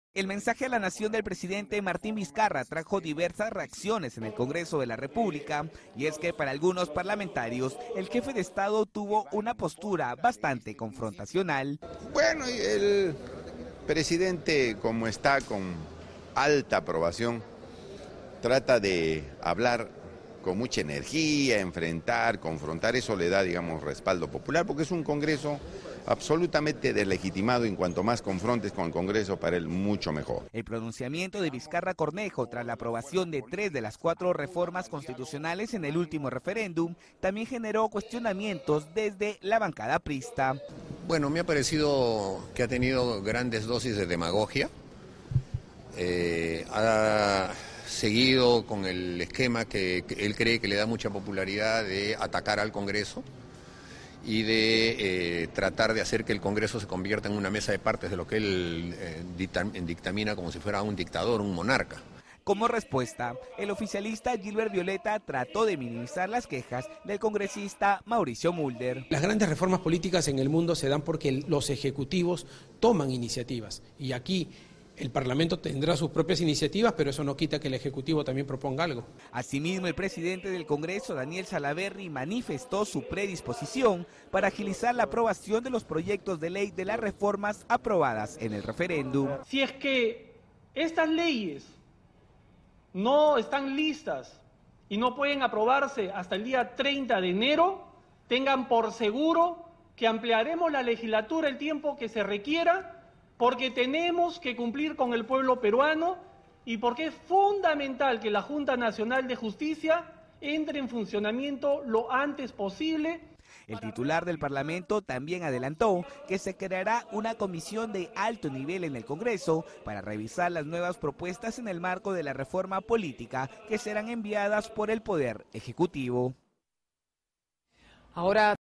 Opiniones divididas en el Congreso tras mensaje a la Nación de presidente Vizcarra
El mensaje a la Nación del presidente Martín Vizcarra trajo consigo diferentes reacciones por parte de los miembros del Congreso, y es que para algunos el mandatario tuvo una postura bastante confrontacional.

El congresista Yonhy Lescano sostuvo que “el presidente como está con alta aprobación, trata de hablar con mucha energía, (busca) enfrentar, confrontar, eso le da mucho respaldo popular, porque es un congreso absolutamente deslegitimado. En cuanto más confrontes con el Congreso para él mucho mejor”.

El pronunciamiento de Vizcarra Cornejo, tras la aprobación de tres de las cuatro reformas constitucionales en el último referéndum, también generó opiniones en los legisladores de la Célula Parlamentaria Aprista.

El parlamentario Mauricio Mulder sostuvo que el mensaje presidencial ha “tenido grandes dosis de demagogia” y ha seguido un esquema “que él (Martín Vizcarra) cree que le da mucha popularidad”.

En respuesta, el congresista oficialista Gilbert Violeta minimizó lo dicho por Mulder y señaló que “las grandes reformas políticas en el mundo se dan porque los (poderes) ejecutivos toman iniciativas, y aquí el parlamento tendrá sus propias iniciativas, pero eso no quita que el Ejecutivo proponga algo.

Quien también se manifestó fue el presidente del Legislativo, Daniel Salaverry, quien dijo tener la predisposición de agilizar la aprobación de los proyectos de ley de las reformas aprobadas en el referéndum y de ser necesario se ampliaría la legislatura el tiempo que sea necesario.